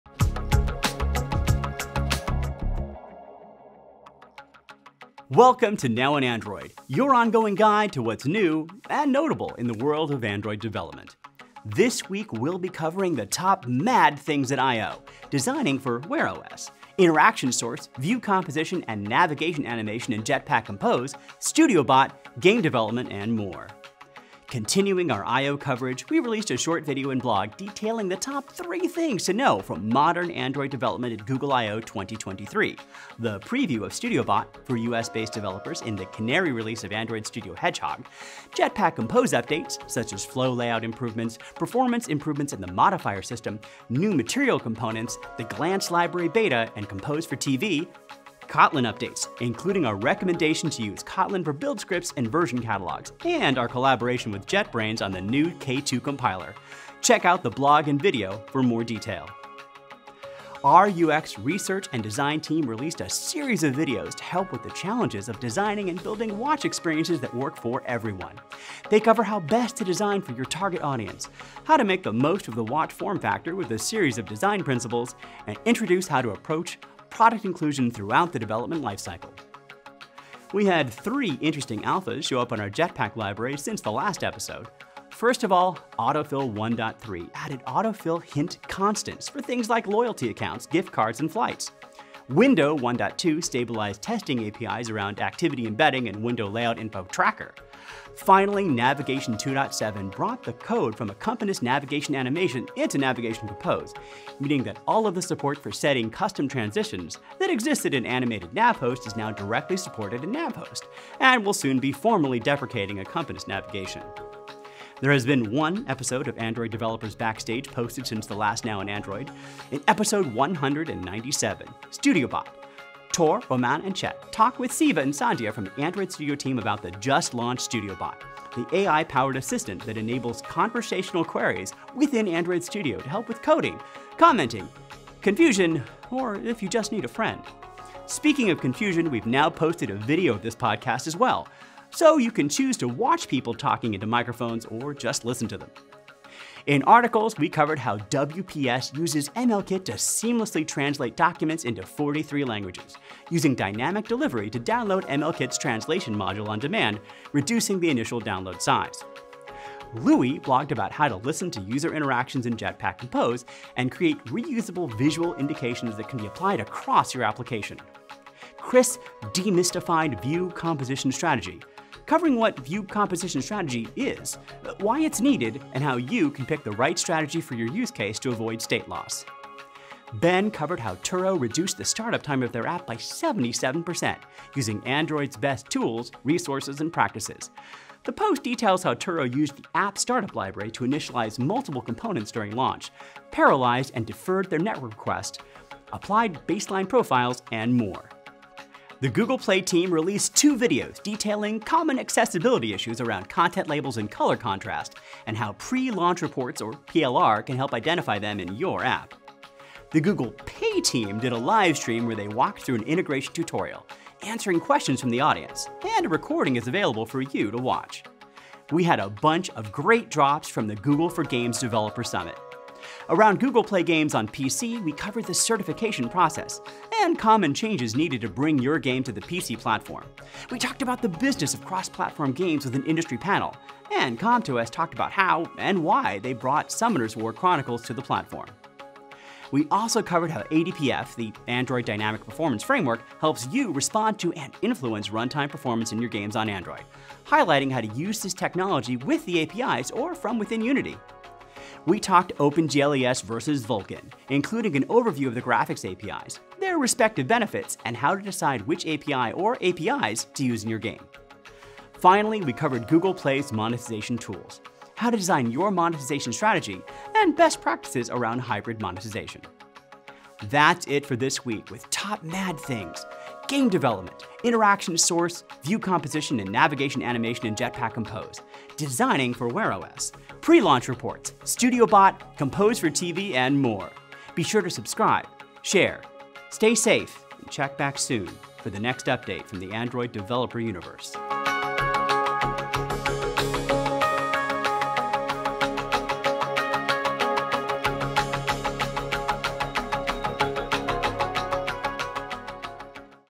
Now in Android: 84 - top MAD things at I/O, designing for Wear OS, InteractionSource, and more!
Welcome to Now in Android, your ongoing guide to what’s new and notable in the world of Android development. Today, we’re covering the top MAD things at I/O, designing for Wear OS, InteractionSource, Studio Bot, ViewComposition, and navigation animation in Jetpack Compose, game development, and more!

Chapters:
0:00 - Introduction
0:24 - Modern Android Development
1:00 - Designing for Wear OS
1:20 - AndroidX updates
1:54 - ADB Podcast - Studio Bot
2:26 - Articles
3:18 - Videos
4:30 - Conclusion

For links to these items, check out Now in Android #84 on Medium